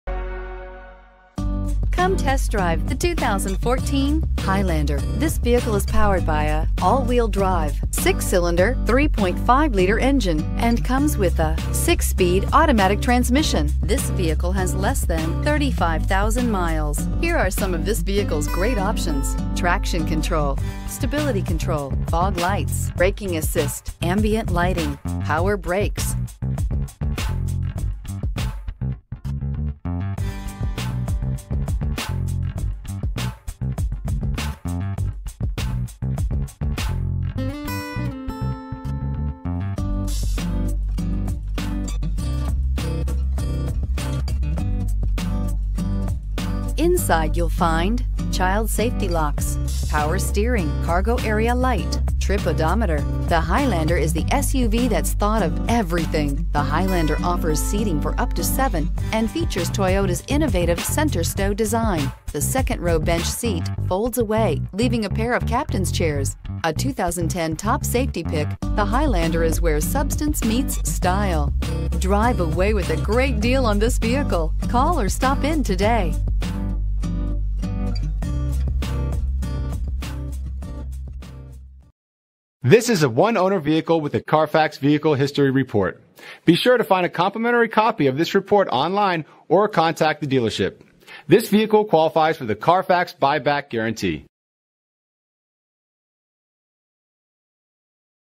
2014 Toyota Highlander Limited for sale in Raleigh NC
2014 Toyota Highlander Limited for sale in Raleigh NC - Stock # 91249B
Year: 2014
Exterior: Blizzard Pearl
Interior: Black
Miles: 31,600
MSRP: N/A
Engine: 3.5L V6 DOHC Dual VVT-I 24V
Transmission: Automatic
Address:
VIN: 5TDDKRFH8ES070346